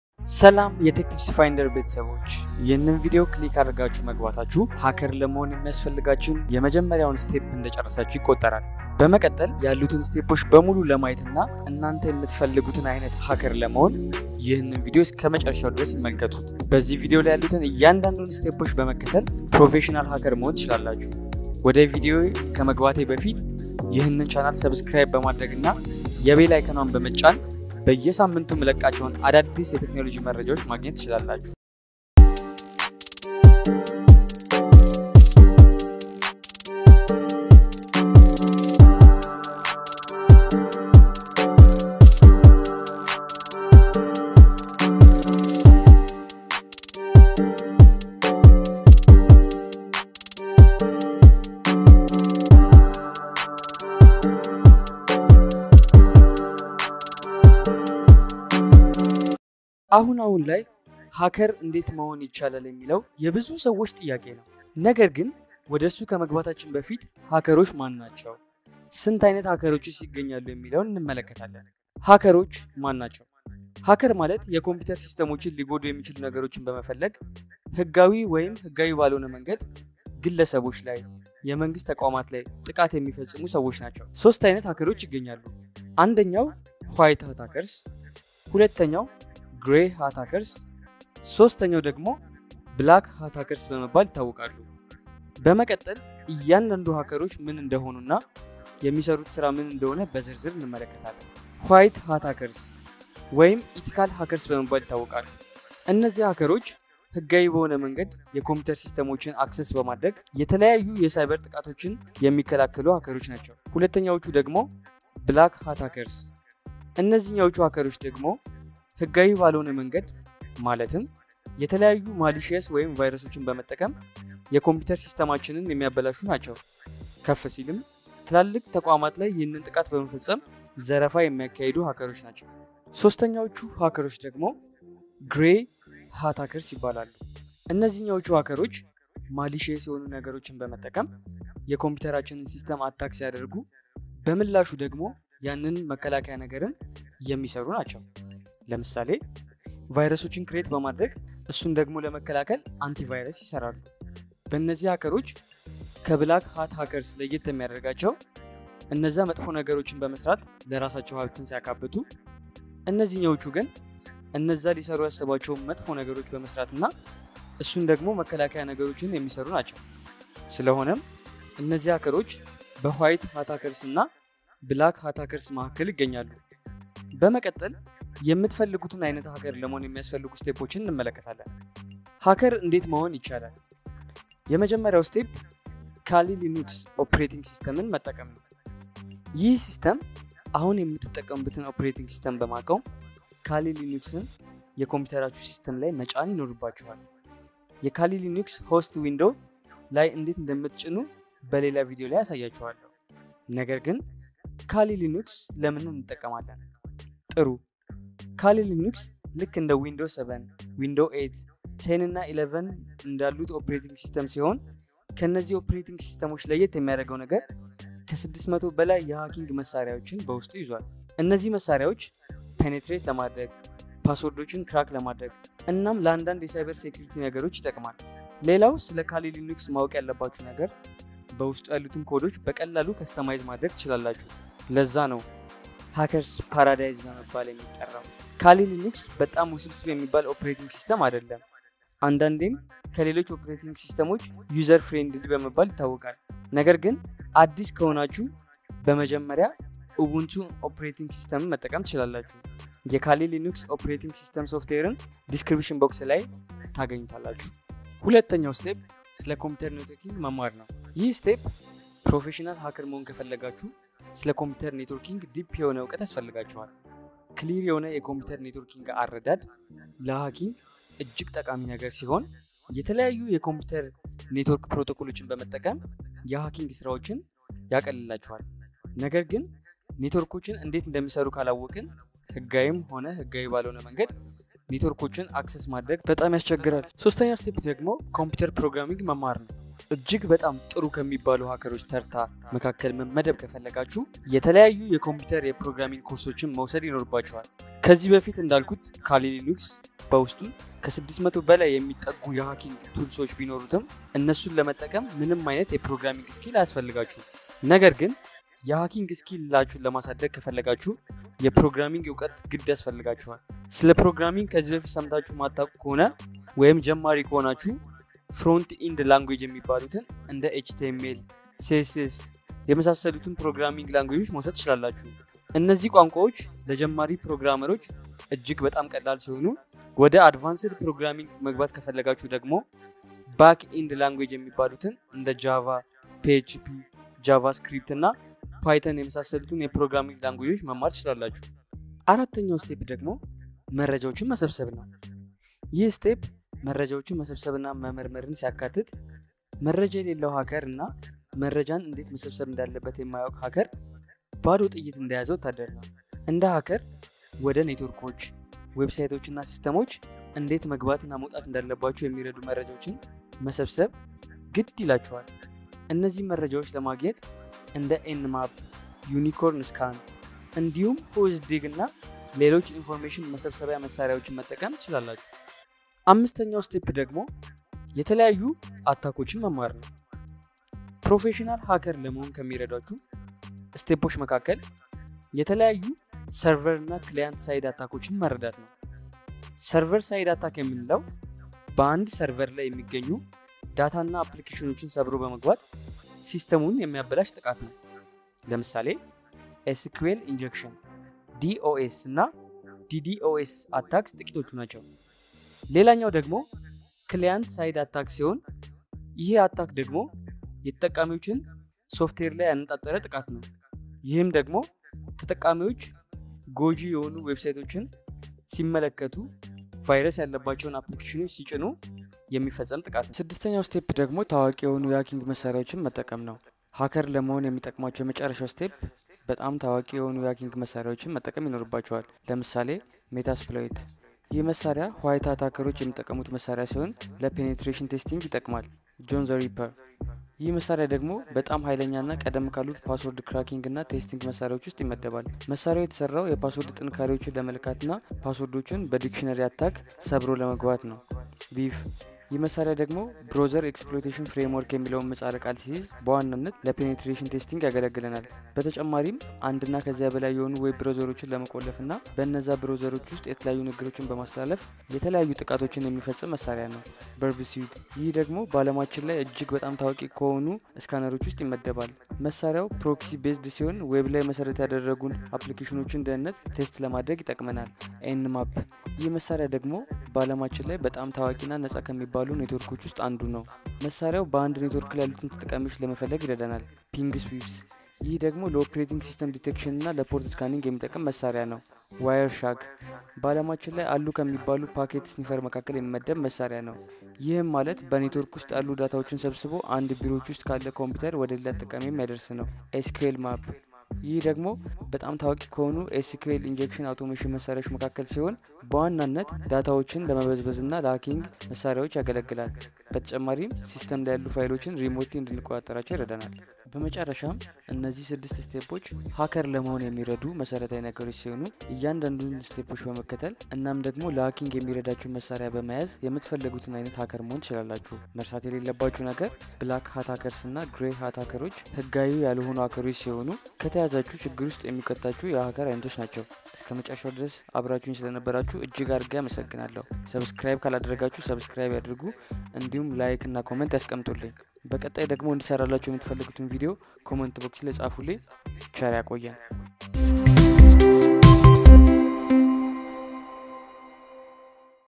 6 ፕሮፌሽናል ሃከር ለመሆን የሚጠቅሙ ነገሮች How To Become A Professional Hacker In (2021) | For Beginners
በዚህ ቪዲዮ ላይ ሃከር ለመሆን የሚያስፈልጉ 6 ነገሮችን አንመለከታለን ። ሃከሮች በ3 የኮፍያ ቀለማት Black Hat Hacker, White Hat Hacker, እና Grey Hat Hacker በመባል ይከፈላሉ። ነገር ግን የትኛውንም አይነት hackerዎች ቢሆኑ የባንክ አካውንት እና password ሰብሮ በመግባት የሚፈልጉትን ጥቃት ማድረስ ይችላሉ።

Learn how to become a hacker in 2021 with these 6 simple steps. These steps are beginner-friendly and will put you on the path to becoming any hacker you want to be.